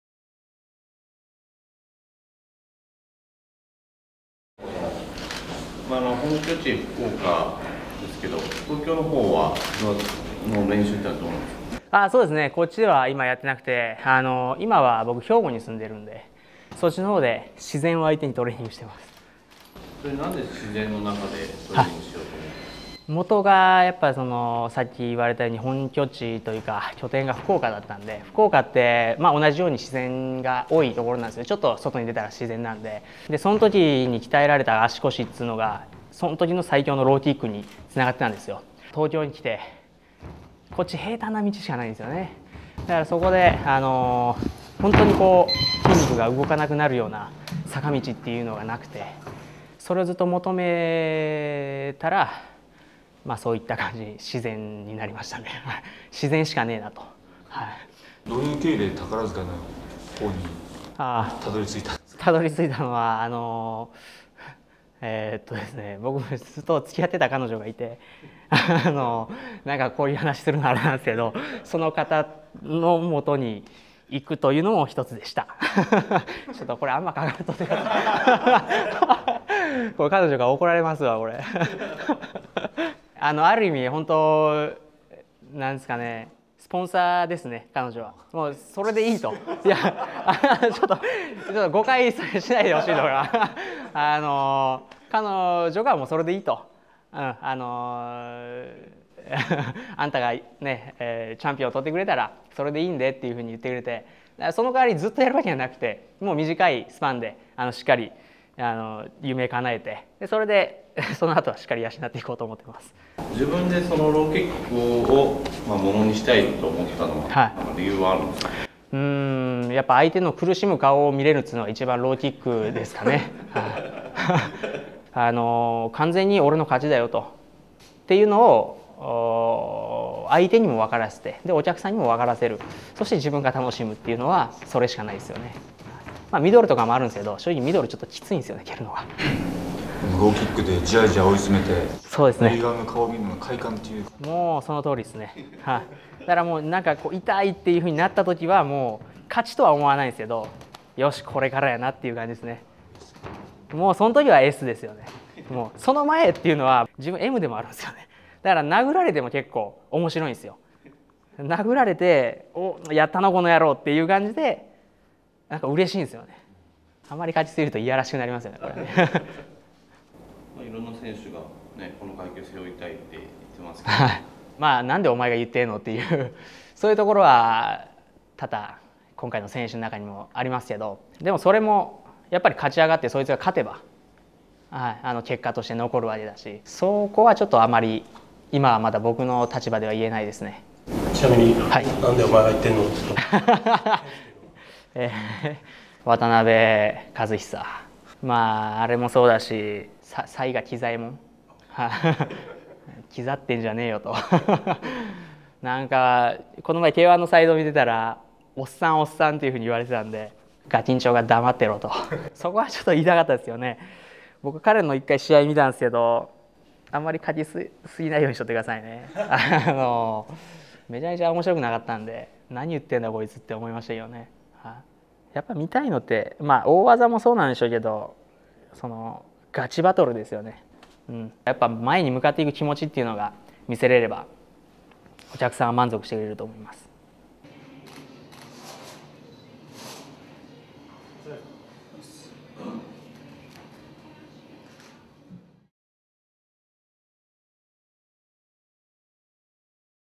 Yuki - Training & Interview - May.2 MAX -63kg
Yuki's interview and training session for K-1 WORLD MAX 2010 -63kg Japan Tournament 1st Round (May.2.2010 / JCB Hall, Tokyo, Japan). Vs. "Kyoken" Yuji Takeuchi!!

5・2『K-1 WORLD MAX 2010 -63kg Japan Tournament 1st Round』JCBホール大会で狂拳竹内裕二と対戦する、裕樹が練習を公開！